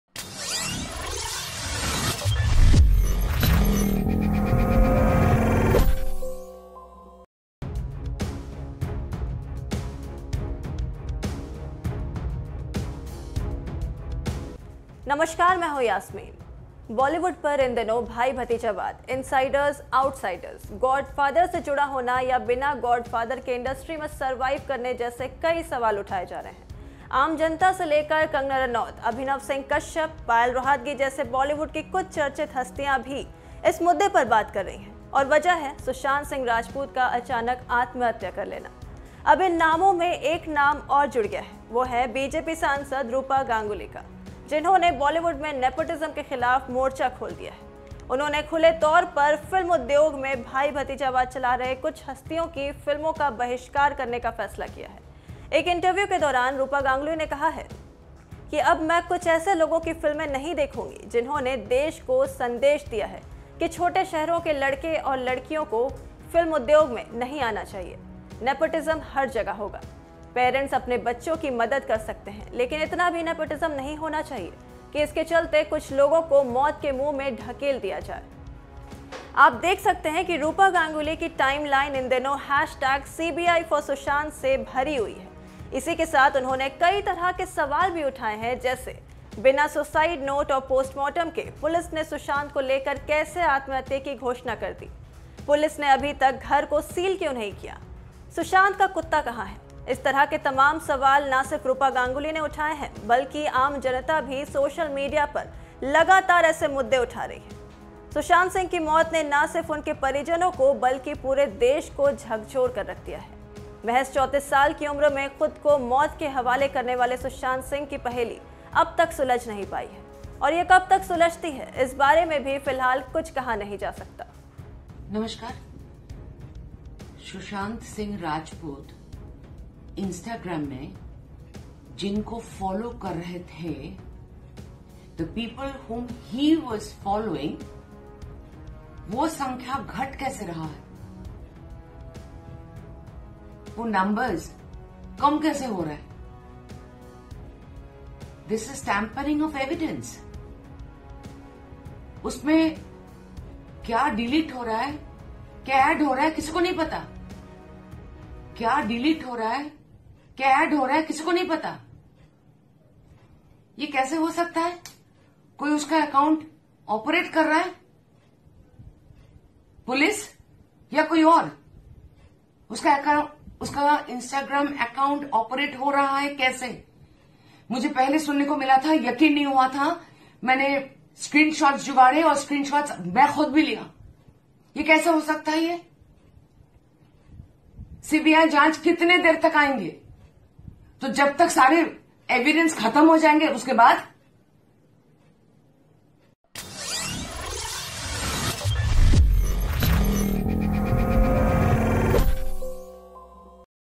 Sushant Singh Case में Roopa Ganguly ने खोला मोर्चा, फिल्मों का करेंगी बहिष्कार
Sushant Singh की मौत को लेकर एक्ट्रेस और बीजेपी सांसद रूपा गांगुली ने मुंबई पुलिस को घेरे में लेते हुए कई सवाल खड़े किए हैं…और इस मामले को लेकर नेपोटिज्म के खिलाफ मोर्चा खोला है…